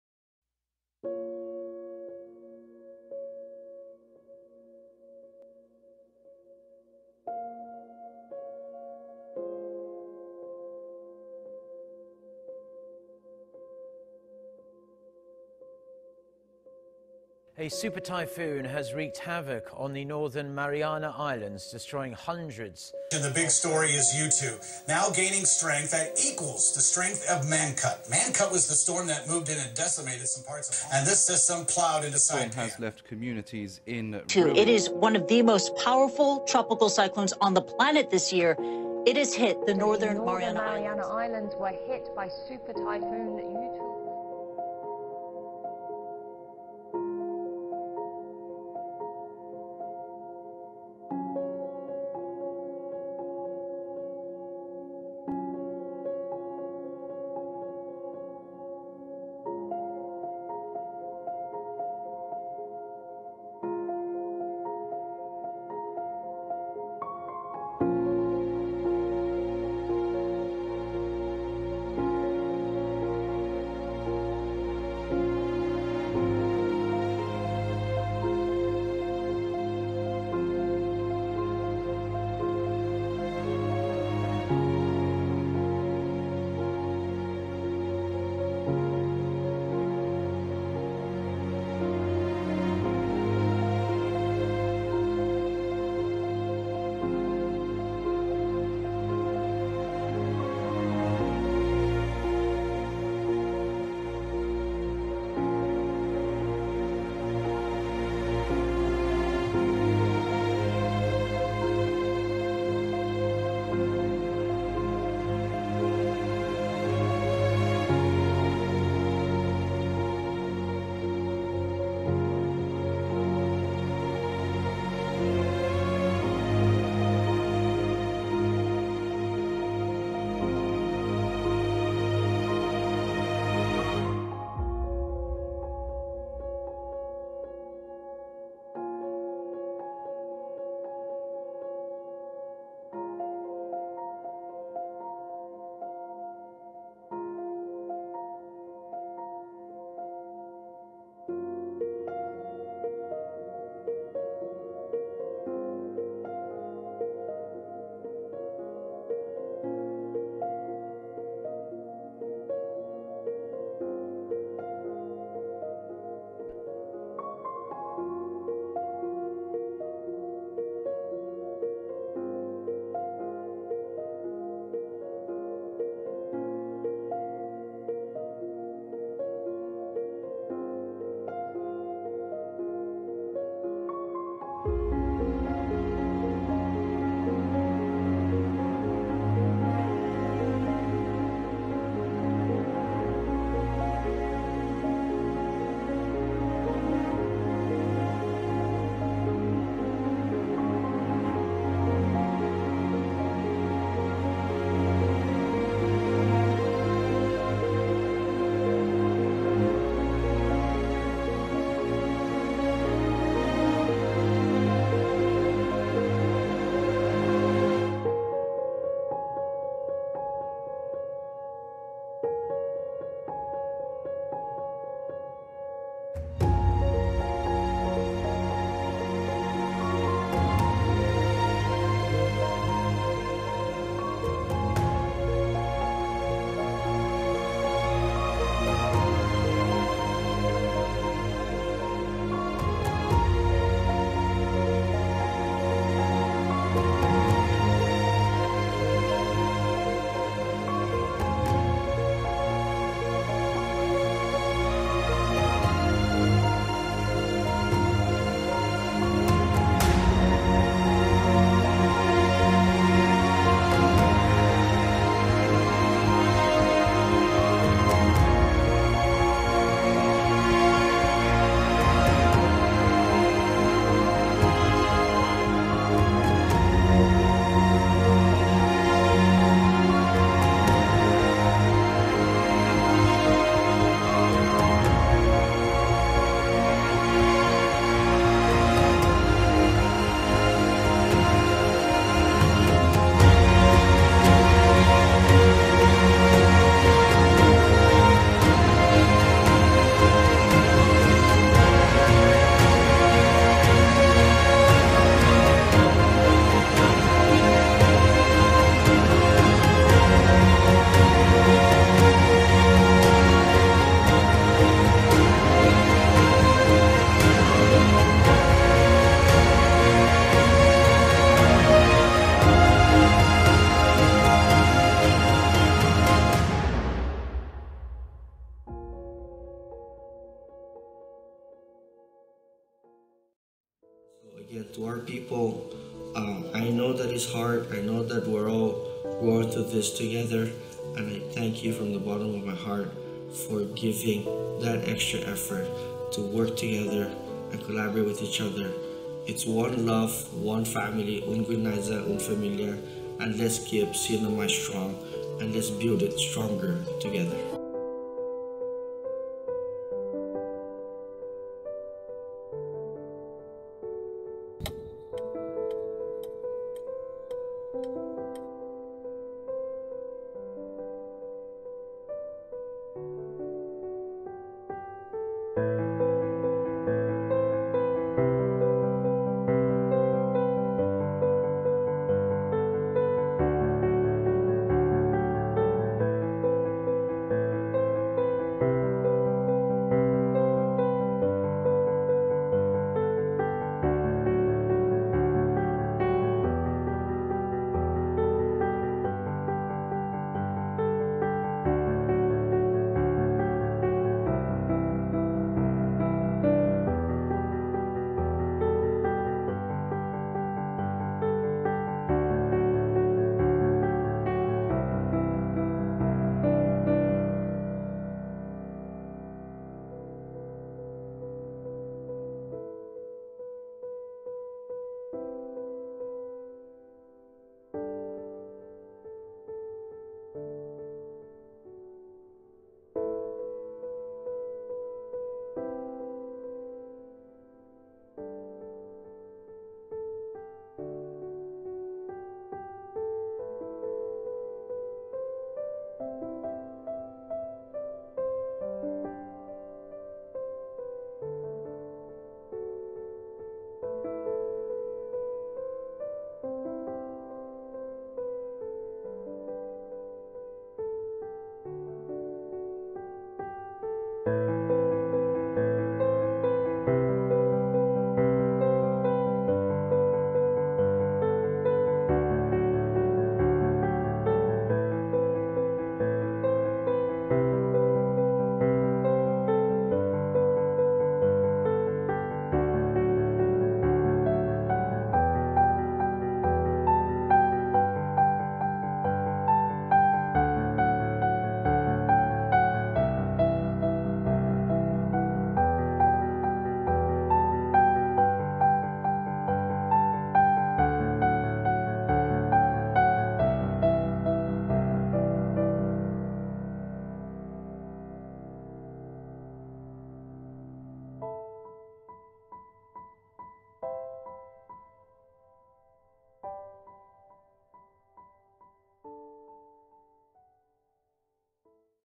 SupertyphoonYutu: The Devastation and the Recovery
Supertyphoon Yutu did devastating damage to the islands of Tinian and Saipan, part of the U.S. Commonwealth of the Northern Mariana Islands.  Images and video of the destruction and the incredible recovery effort.